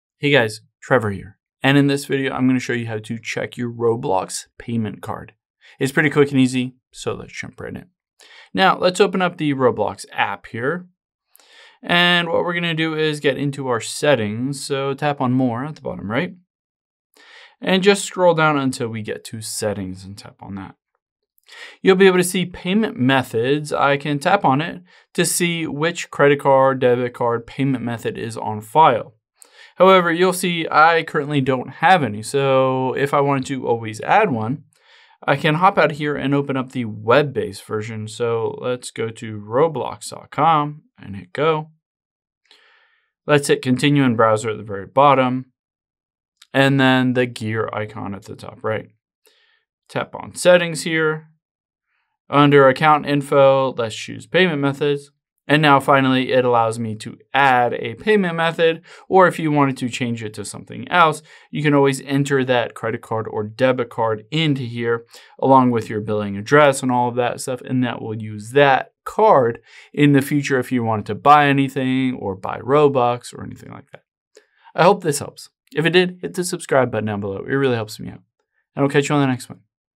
How To Check Roblox Payment Card
Here's how to check which payment cards are on your Roblox account so you know what cards will be charged if purchases are made in the shop.